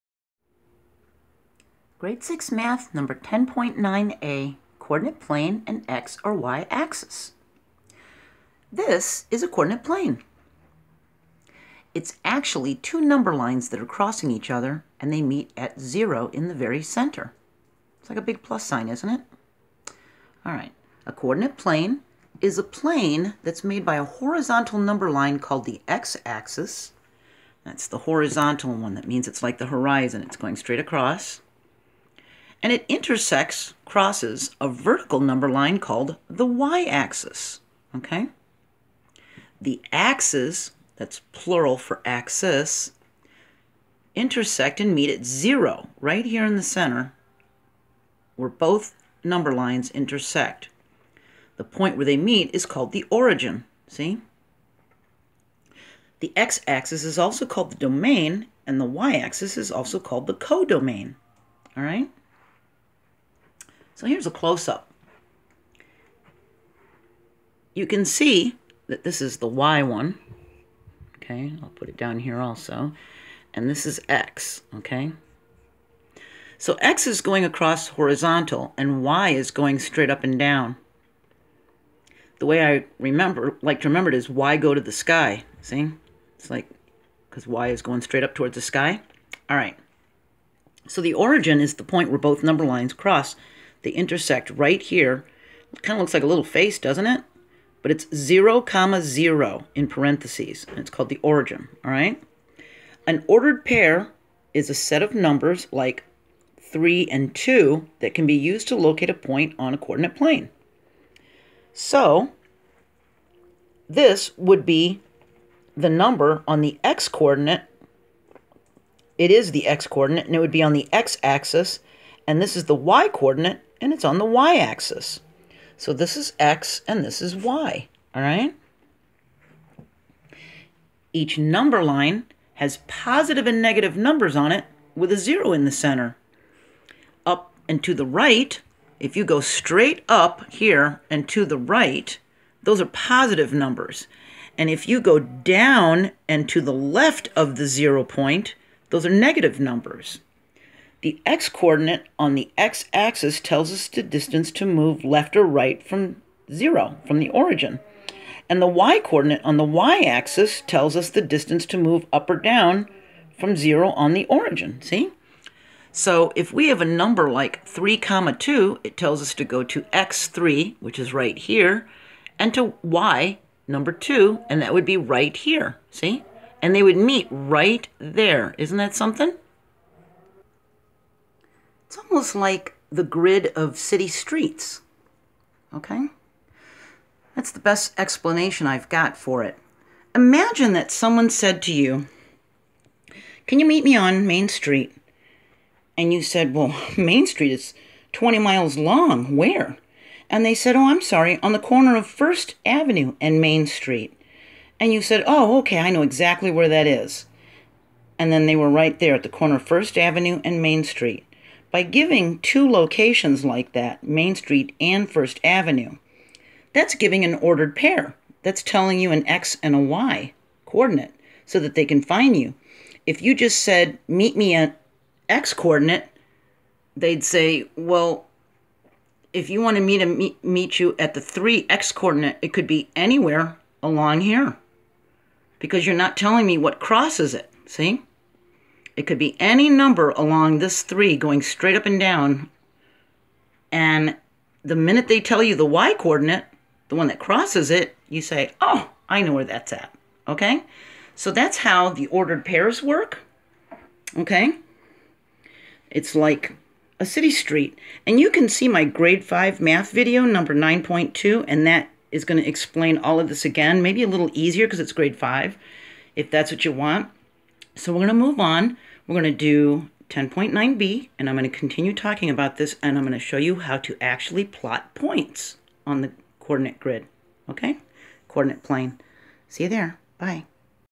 Grade 6 Math #10.9a, Coordinate Plane - Part 1, X and Y axis
An explanation of ordered pairs, origin, x coordinates, y coordinates, domain, co-domain, and the coordinate plane as a graph. The next few videos prepare us to graph inequalities on a coordinate plane.  #10.9a

Grade 6 Math #10.9b, Coordinate Plane - Part 2,
Quadrants and Plotting